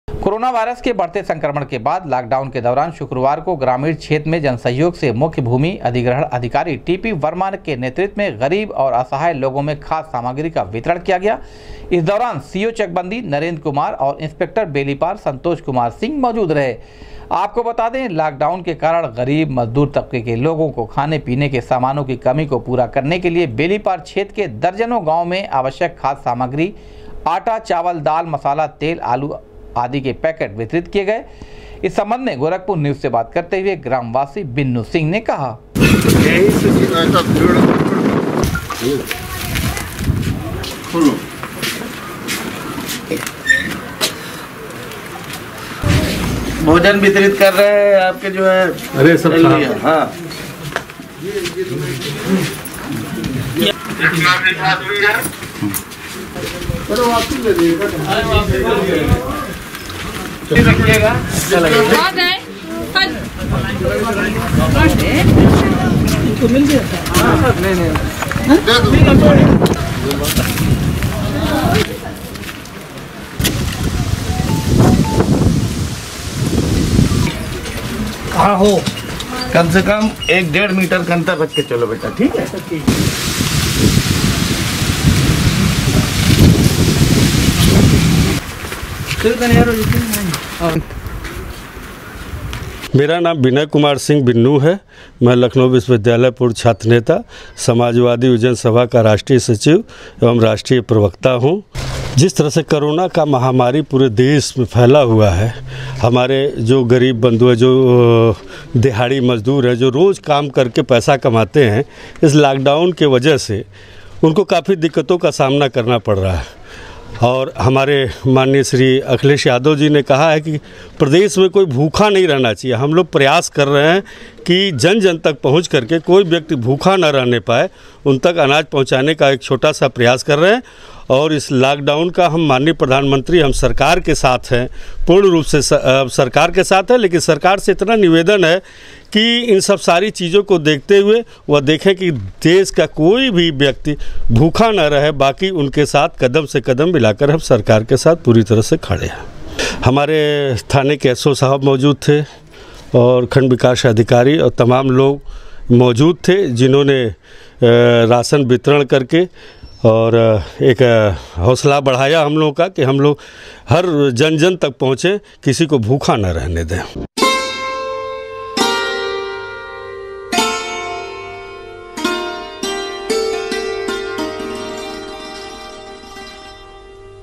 ग्रामीण क्षेत्र में जनसहयोग से मुख्य भूमि अधिग्रहण अधिकारी टी पी वर्मा के नेतृत्व में गरीब और
ग्रामीण क्षेत्र में जनसहयोग से मुख्य भूमि अधिग्रहण अधिकारी टी पी वर्मा के नेतृत्व में गरीब और असहाय लोगों में खाद्य सामग्री का किया गया वितरण, सोशल डिस्टेंस का किया गया पालन।